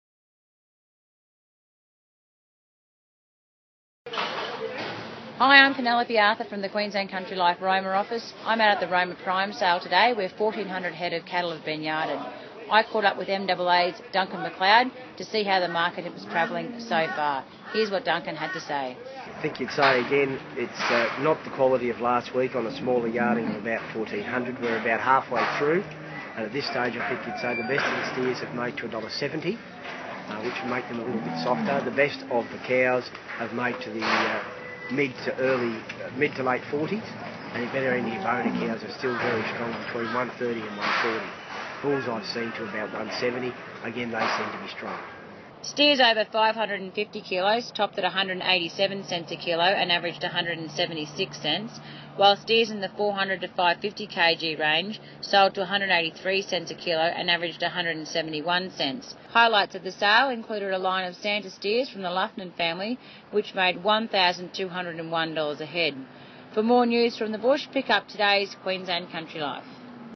Roma steers realise 187c
QCL Roma reporter Penelope Arthur went to the August 2, Roma prime sale to find out how the market was travelling.